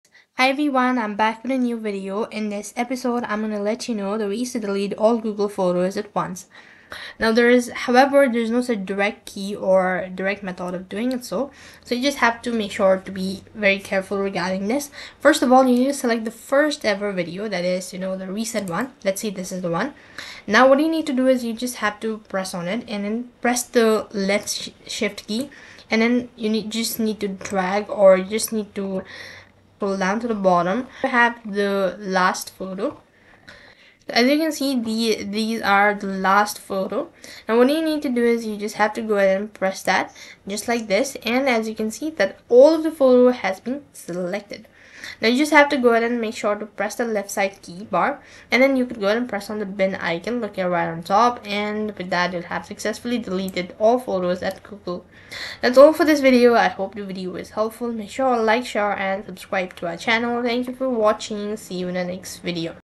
How To Delete All Google Photos At Once !
In this video, you'll learn how to delete all your Google Photos at once. While Google Photos doesn't offer a direct option to delete all photos in one click, there is an effective way to do it using a simple selection technique. Here's how to get it done.

Start by opening Google Photos in your browser. First, select the very first photo in your library by clicking on it. After selecting the first photo, hold down the "Shift" key on your keyboard. While holding down the "Shift" key, scroll down to the very last photo in your Google Photos library. Click on this last photo while still holding the "Shift" key. This action will select all the photos between the first and last image, effectively selecting your entire photo library.

Once all photos are selected, look for the trash bin icon at the top of the screen. Click on it, and all the selected photos will be moved to the bin, effectively deleting them from your Google Photos account. Be cautious with this method, as it will permanently delete your photos after 60 days unless restored from the bin.

Timestamps:
0:00 Introduction
0:06 Selecting the first photo
0:16 Using "Shift" to select all photos
0:40 Deleting selected photos using the bin icon
0:53 Conclusion